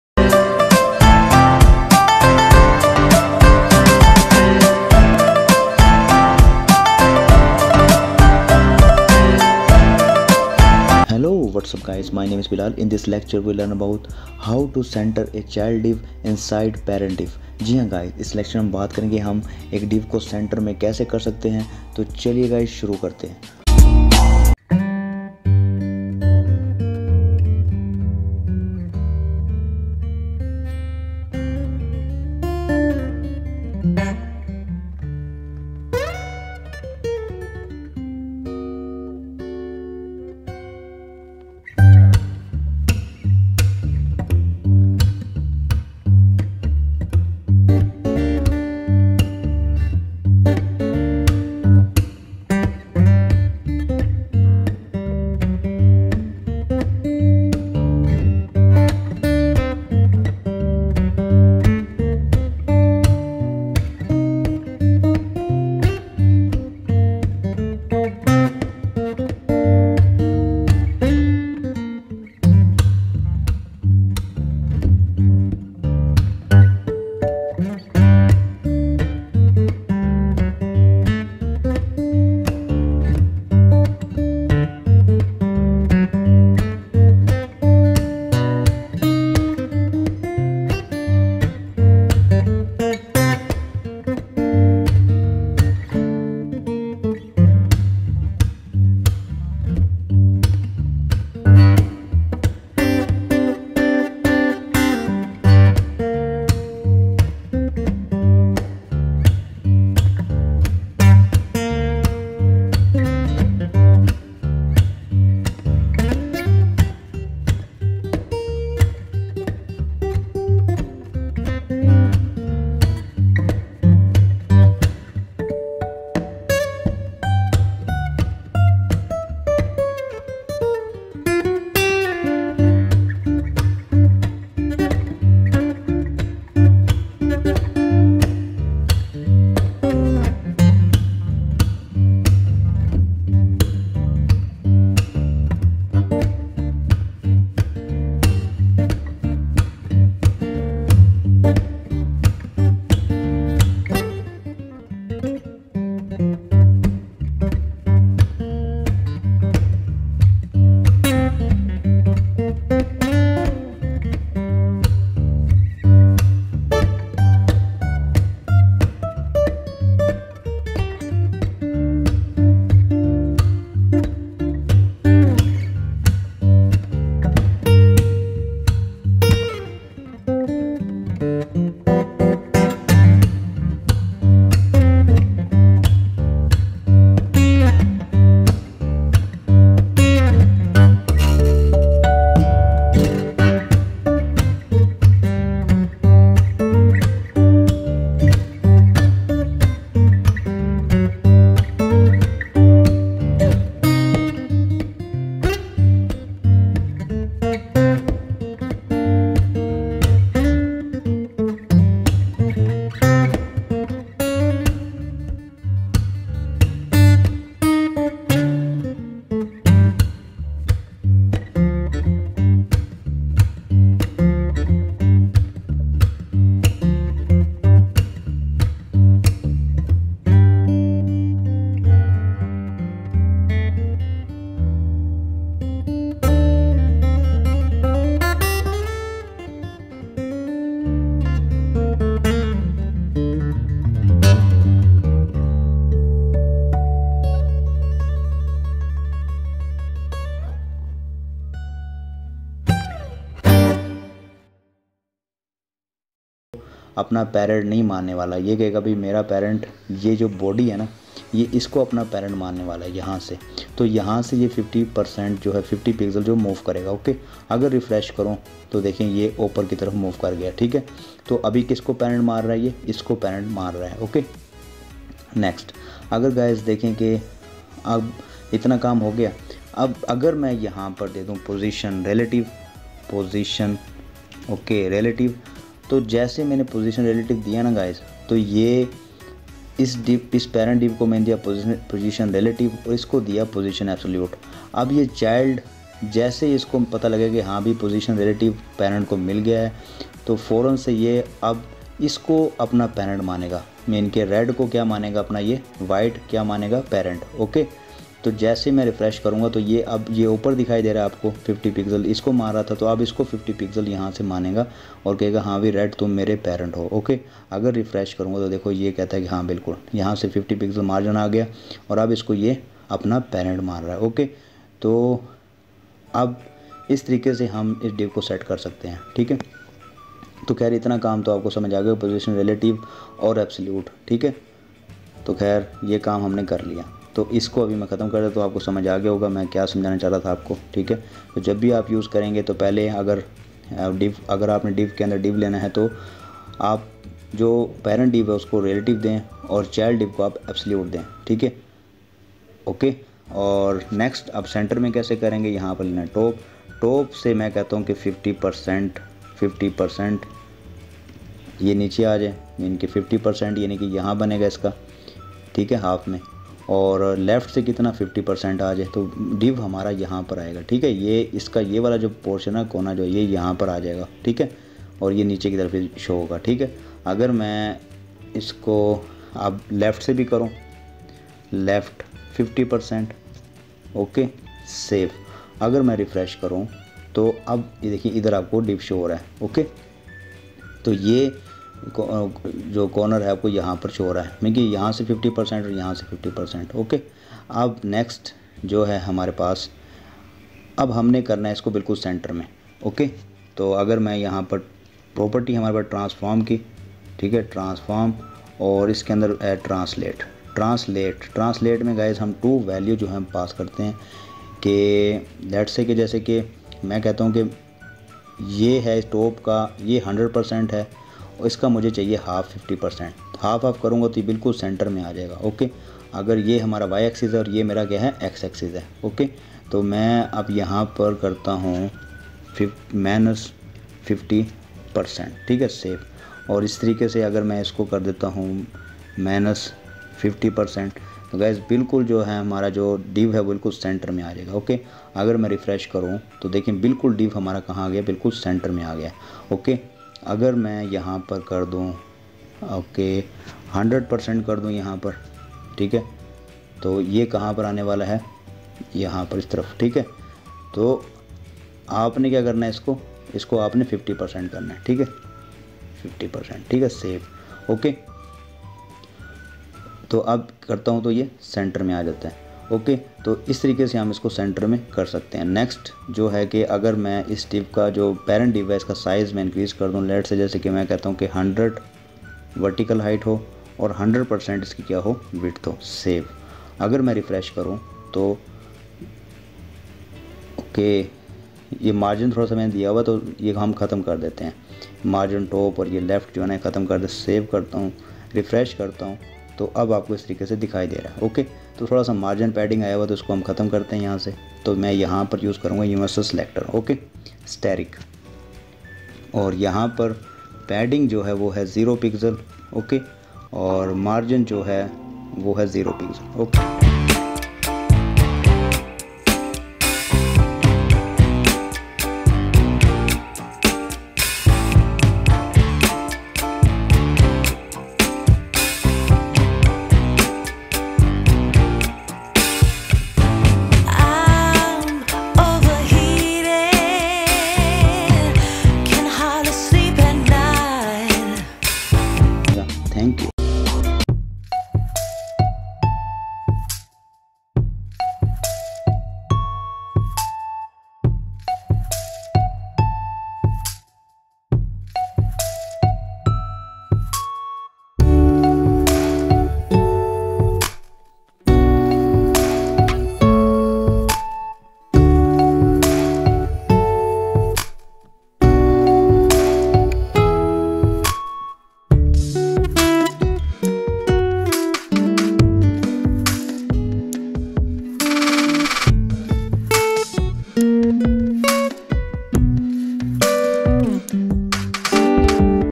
26- HTML5/CSS3 Tutorials in Urdu - Hindi I How to center a child Div inside another parent Div
Welcome all in this lecture , we learn about how to center a child DIV inside another parent DIV..